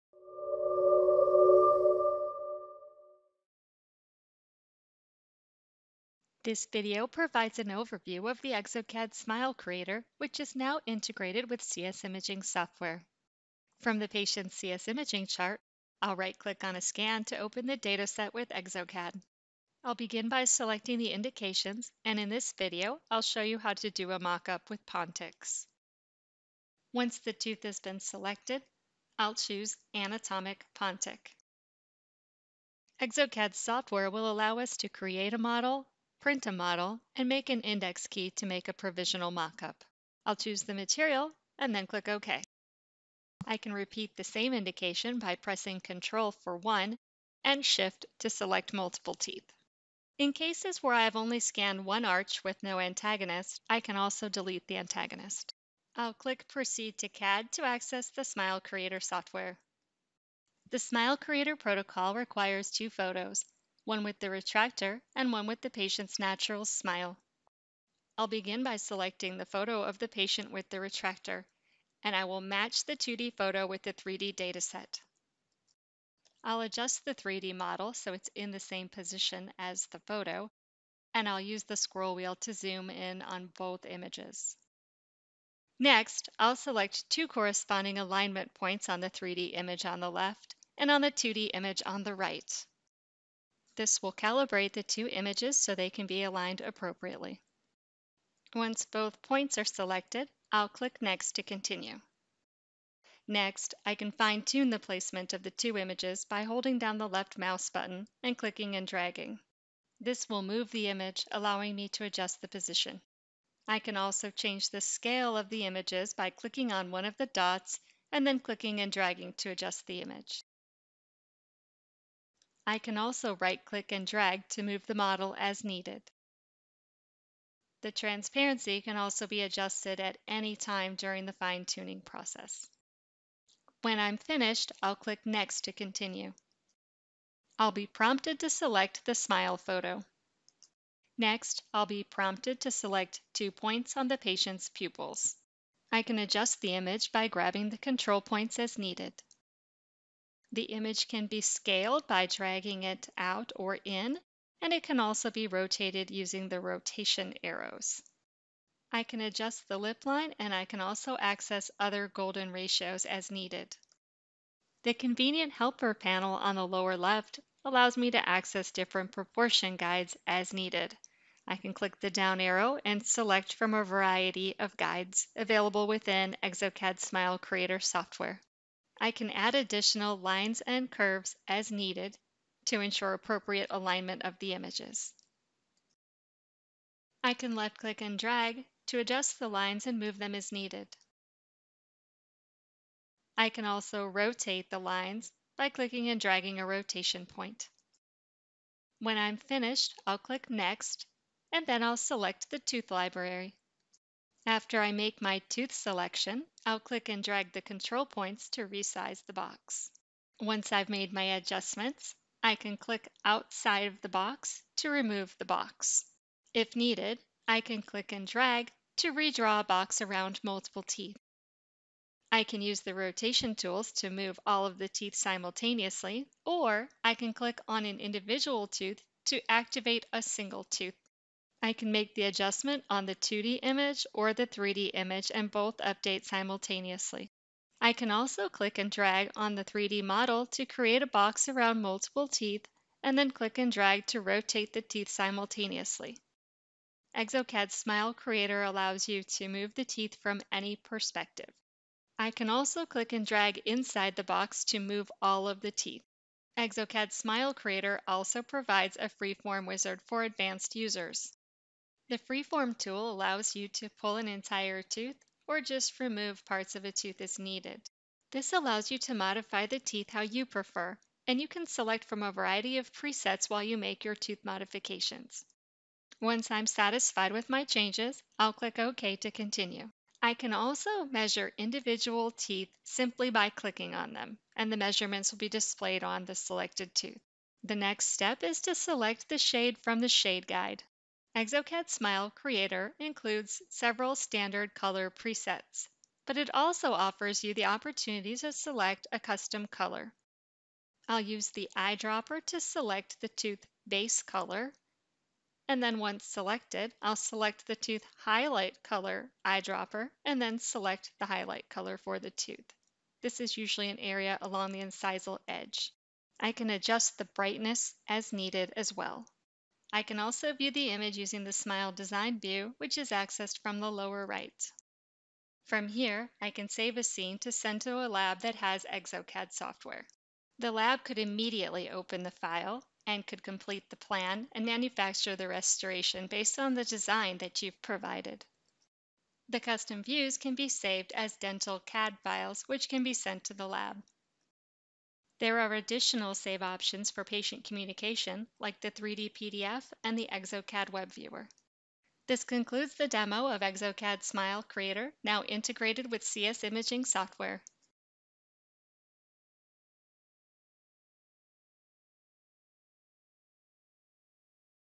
Exocad Smile Creator (moduł DSD) zintegrowany ze skanerami wewnątrzustnymi CS 3600 oraz CS 3700
Teraz Exocad Smile Creator (moduł DSD) zintegrowany z naszymi skanerami wewnątrzustnymi CS 3600 oraz CS 3700.  ❤️❤️❤️

Integracja zwyciężających w wielu porównaniach skanerów Carestream z najbardziej kompleksowym oprogramowaniem CadCam pozwoli zarówno lekarzowi jak i technikom jeszcze lepiej projektować DSD w 3D.
Skan 3D nałożony na projekty 3D w Exocad pozwalają na większą precyzję i przewidywalność poszczególnych etapów planu leczenia.

Rozwijaj swój Gabinet dzięki skanerom wewnątrzutnym Carestream oraz oprogramowaniu Exocad Smile Creator.

W kilka chwil pokaż Pacjentowi, jak zmieni się ich życie, a nam pozwól pokazać to Tobie.
👉Chcesz się dowiedzieć więcej?
Umów się na spotkanie w tygodniu, lub w wybraną sobotę sobotę